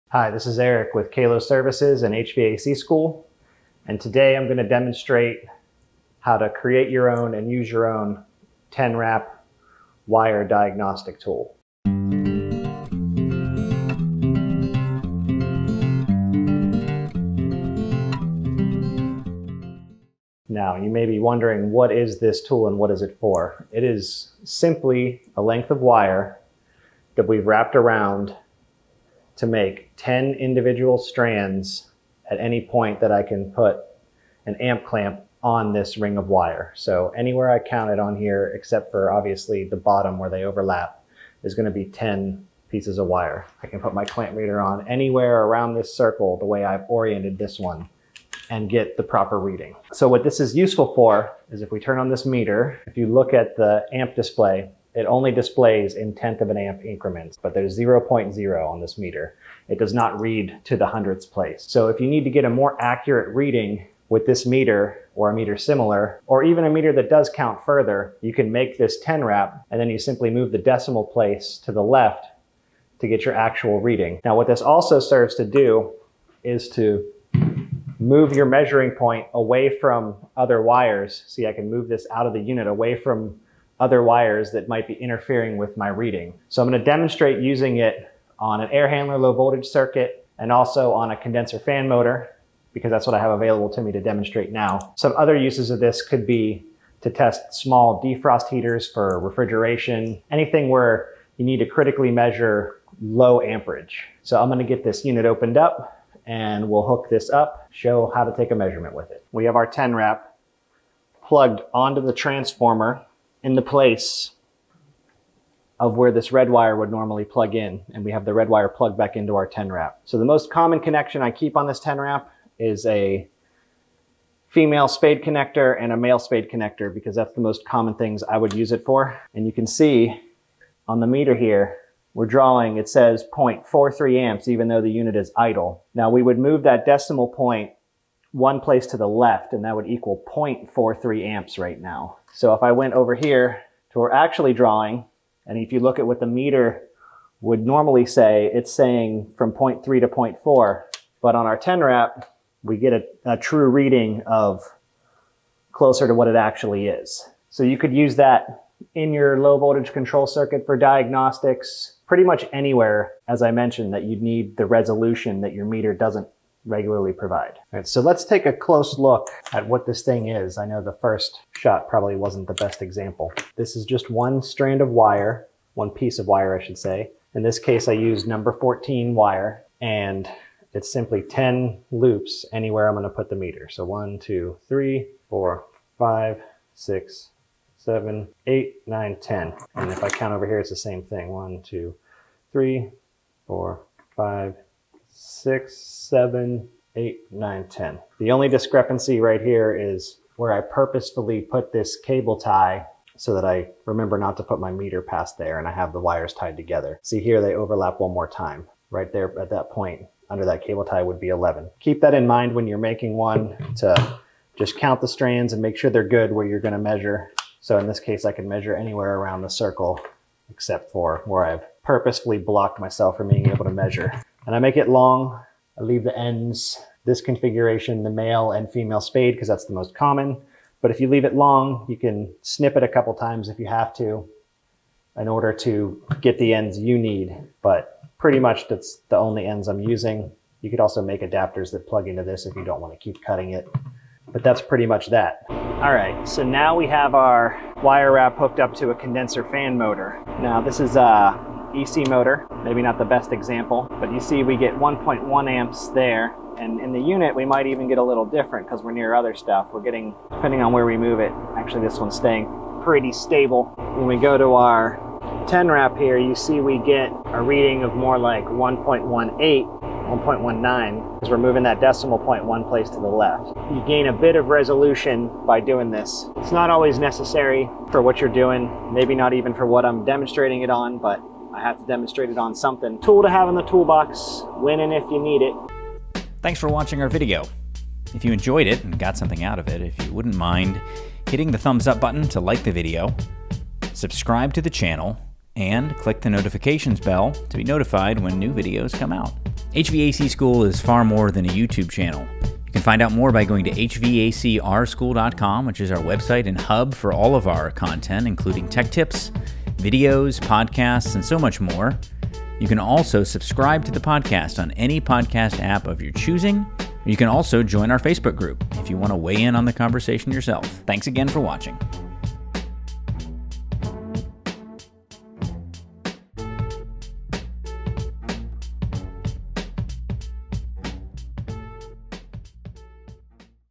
How to Easily Measure Low Amperages in AC
In this video, Eric Mele shows how to how to easily measure Low Amperages in AC. He uses a 10-wrap wire rig to amplify low voltage and condensing fan signals for more accurate current measurements.

To make a 10-wrap rig, you simply take one strand of wire and wrap it to make 10 individual strands in a loop. You can put an ammeter clamp anywhere on it and get a more precise amperage measurement than your meter allows by using a little bit of math.

When you measure the amps on a rig with 10 wraps of a single wire, the amps of the wire itself are multiplied by 10. Thus, when you apply your ammeter clamp to the 10 wire rig, all you have to do is take the measurement and move the decimal point to the left to determine the amperage of a single wire. The 10-wrap rig also isolates the wire and makes you less likely to pick up amperage from other nearby wires.

For example, you could check the amperage on a condenser fan motor and yield 1.1 amps. When you use a 10-wrap rig, you will get a measurement like 11.8 for the wire with 10 loops. You move the decimal point over and get an amperage reading of 1.18 for a single wire. That's a lot closer to 1.2 than 1.1, and that extra precision can be quite helpful.

We use #14 wire with male and female spade connectors in this video.